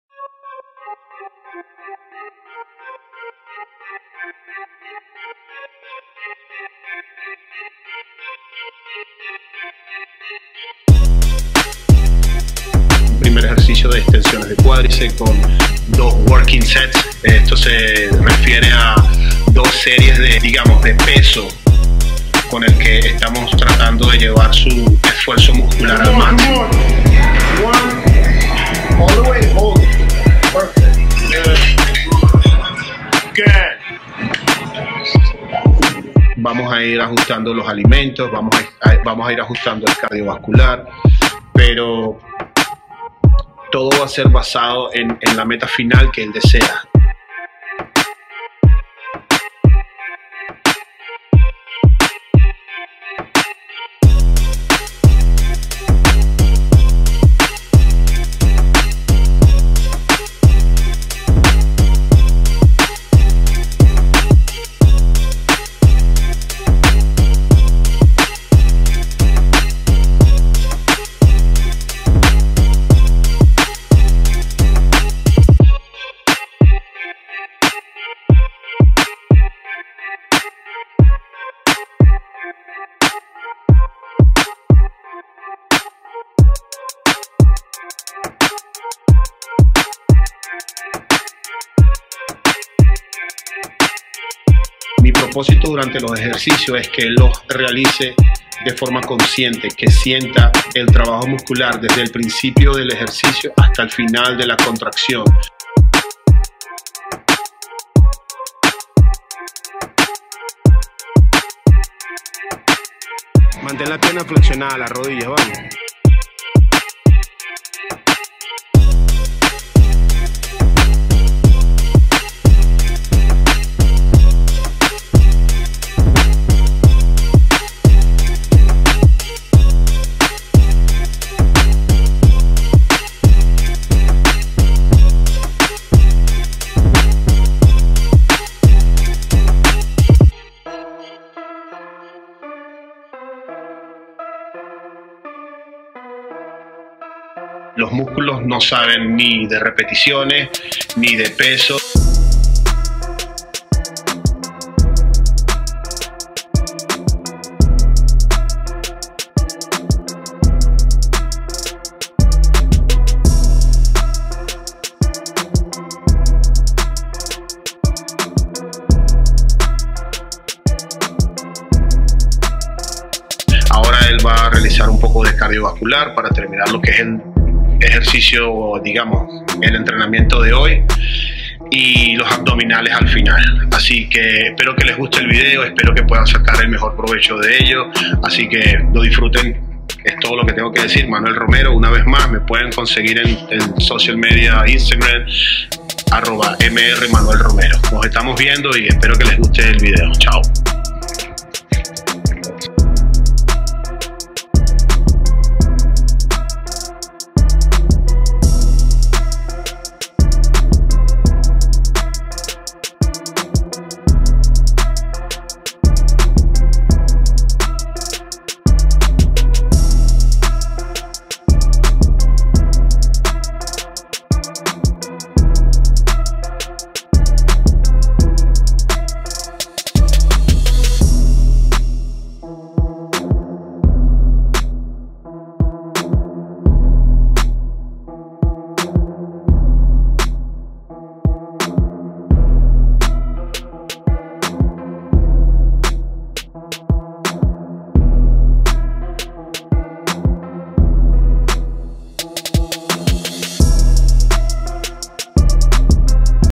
FIT2020 VLOG 2- BEST LEG WORKOUT FOR BEGINNERS
VLOG 2 Best Leg workouts

Trainer: Manuel Romero   IG: @mrmanuelromero
Model: Jorge Alberto  IG: @iamjoral